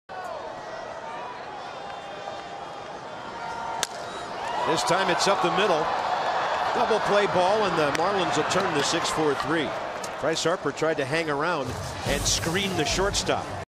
MIA@WSH: Koehler induces DP, escapes jam in 3rd
Tom Koehler gets Ryan Zimmerman to ground into a 6-4-3 double play with two runners on, ending the 3rd inning